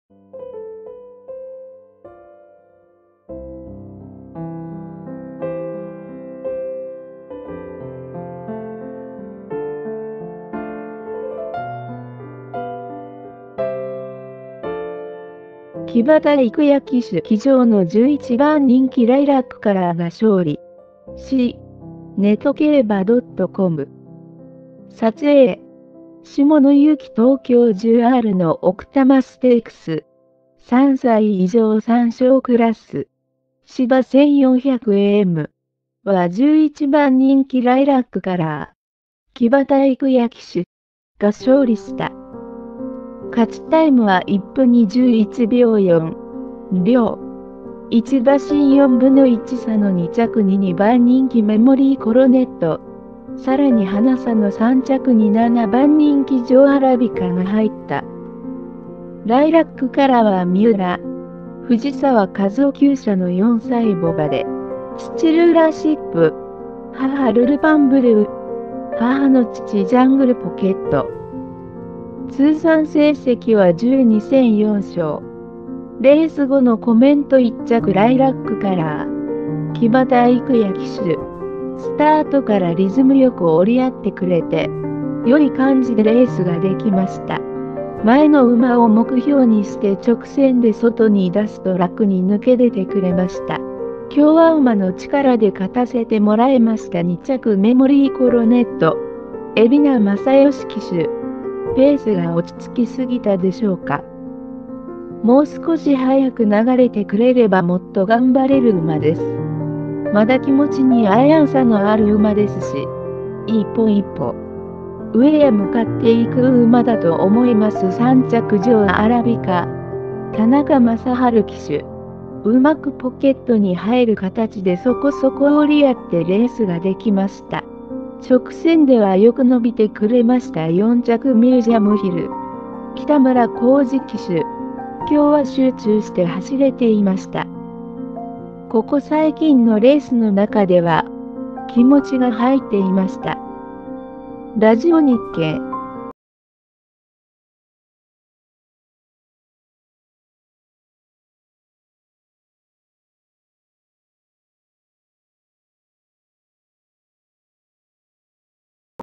【奥多摩S】(東京) 伏兵のライラックカラーがOP入りを果たす | 競馬ニュース - 【速報】!
木幡育也騎手騎乗の11番人気ライラックカラーが勝利(c)netkeiba.com、撮影：下野雄規 　東京10Rの奥多摩ステークス(3歳以上3勝クラス・芝1400m)は11番人気ライラックカラー(木幡育也騎手)が勝利した。勝ちタイムは1分21秒4(良)。1馬身1／4差の2着に2番人